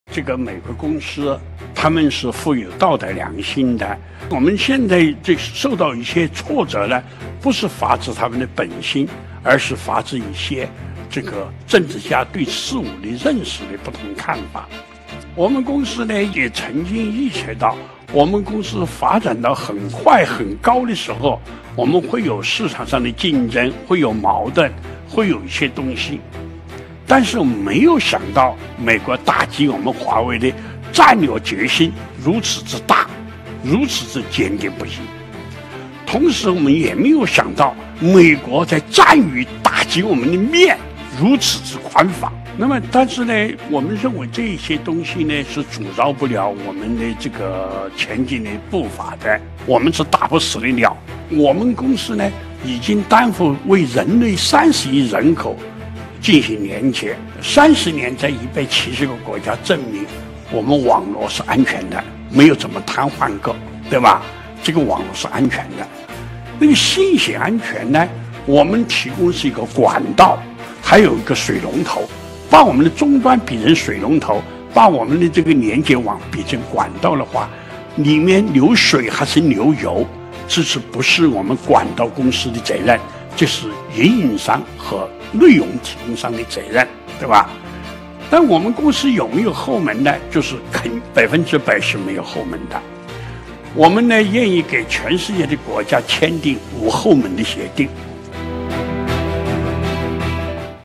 【短片】【中國技術自強不息】任正非：我們是打不死的鳥！華為百分之百沒有後門 願與全世界國家簽無後門協定
//任正非理直氣壯，客觀又冷靜分析目前局勢。一齊俾LIKE，支持中國技術！//